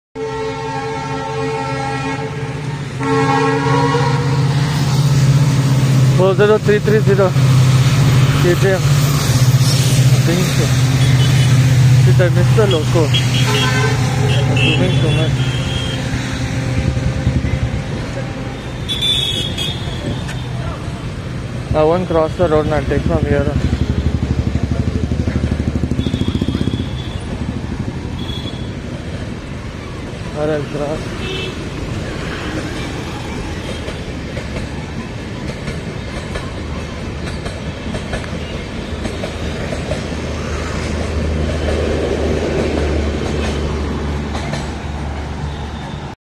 06524 KSK clone spl | Kjm wdp4d 40330 ( not sure of number ) | Captured Today
7/10/2020 18:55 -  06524 Humsafar Clone special (Karnataka Sampark Kranti schedule)

Since some guys requested 5 min extra for answering some questions Sir gave, else he would have left us 2-3 min earlier... RAN LIKE MAD on road but still reached riad opp side of gate as loco was about to pass .. slight light on loco number looked like it was *Kjm wdp4d 40330* ( not sure of number ) with *06524 KSK clone spl*... Sadly as I was running I wss also looking ahead for some PPL who I should not clash with coming opposite side as well as trying to increase lens focus of camera ...which again was not ready to increase easily in this phone ..I think I have to wait at a place only then it will adjust lens..not sure if with moving video it will adjust..m as a result kept in bit zoom Incase I don't make it close enough to that train... And recorded initially without looking at camera and concentrated on reaching gate quickly.mas a result It didn't capture loco due to zoom and facing upwards😑😑😑